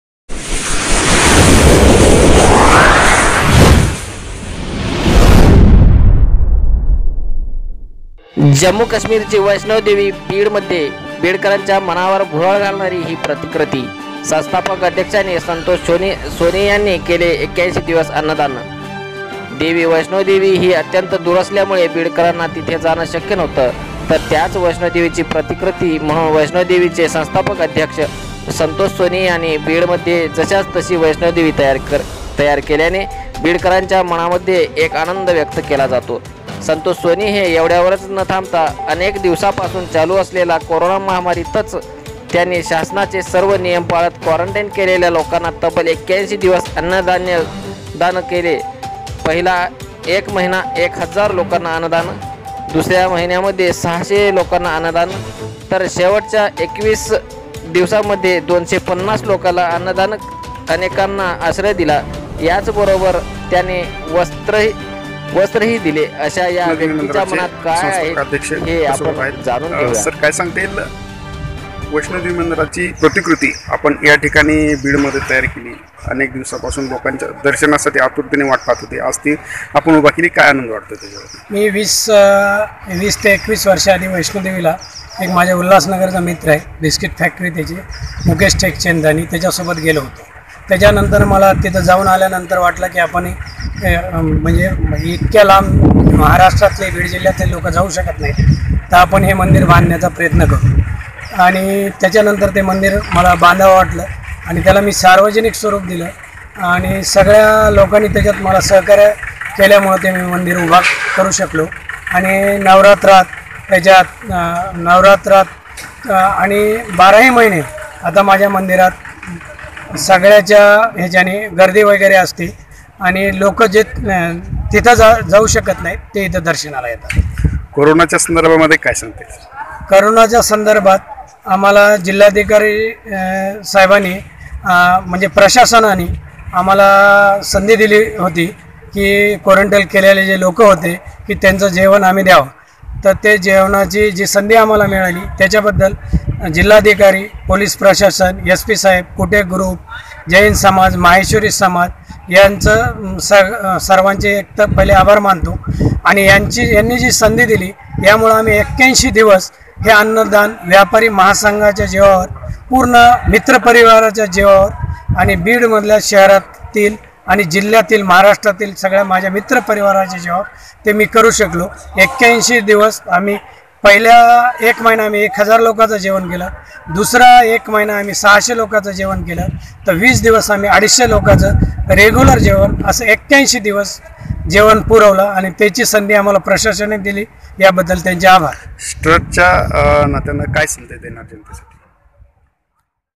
जम्मू काश्मीरची वैष्णवी देवी बीडमध्ये
*बीडकरांच्या मनावर भुरळ घालणारी हि प्रतिक्रती*
*संस्थापक अध्यक्षांनी संतोष सोनी यांनी केले 81 दिवस अन्नदान*

देवी वैष्णोदेवी ही अत्यंत दूर असल्यामुळे बीडकरांना तिथे जाणं शक्य नव्हतं तर त्याच वैष्णोदेवी ची प्रतिकृती म्हणून वैष्णोदेवी चे संस्थापक अध्यक्ष संतोष सोनी यांनी बीडमध्ये जशास तशी वैष्णोदेवी तयार केल्याने बीडकरांच्या मनामध्ये एक आनंद व्यक्त केला जातो संतोष सोनी हे एवढ्यावरच न थांबता अनेक दिवसापासून चालू असलेला कोरोना महामारीत त्यांनी शासनाचे सर्व नियम पाळत काँरन्टाई केलेल्या लोकांना तब्बल ८१ दिवस अन्नदान केले पहिला एक महिना 1000/ (एक हजार) लोकांना अन्नदान दुसऱ्या महिन्यामध्ये600/ (सहाशे) लोकांना अन्नदान तर शेवटच्या एकवीस दिवसांमध्ये 250/ (अडीचशे) लोकांना अन्नदान अनेकांना आश्रय दिला याबरोबरच त्यांना वस्त्रही दिले अश्या या व्यक्तीच्या मनात काय आहे हे आपण जाणून घेऊया